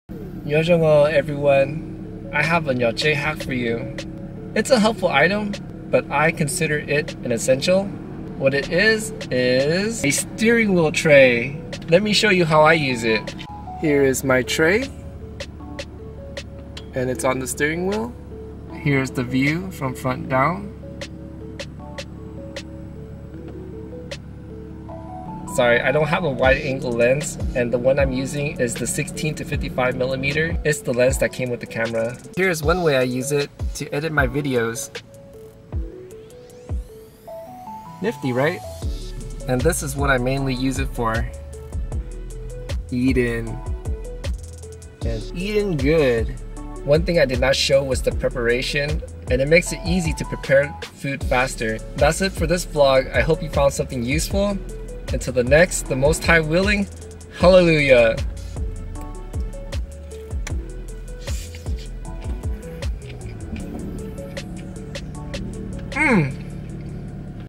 VLOG 023 - Steering Wheel Tray, Nyob Tsheb (Car Life) Hack
Nyob zoo os. Here's a nifty item for Nyob Tsheb. The steering wheel is a great accommodation for video editing, preparing food, projects that require a small table, and eat on. Here are some of my usage. Enjoy! 😊💯👍

Filmed With:
Camera: Sony ZV-E10
Mic: Sony ZV-E10 Internal Mic

© Copyright by El Yaj Vlogs (☞ Do not Reup)
© Bản quyền thuộc về El Yaj Vlogs (☞ Đừng Tải lại)